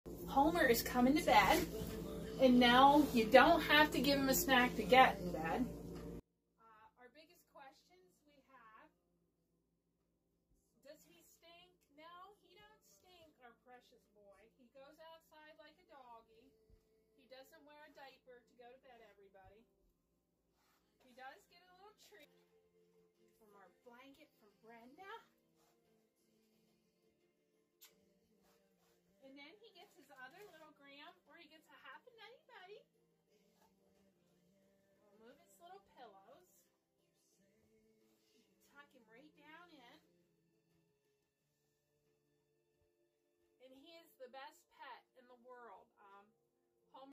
Pet hog 'Homer' tucked into bed by owner
If you ever wondered how a pet pig goes to bed, a Pennsylvania woman has the answer.

Valerie Cranmer and her pet pig Homer, posted this video, showing Homer's bedtime routine.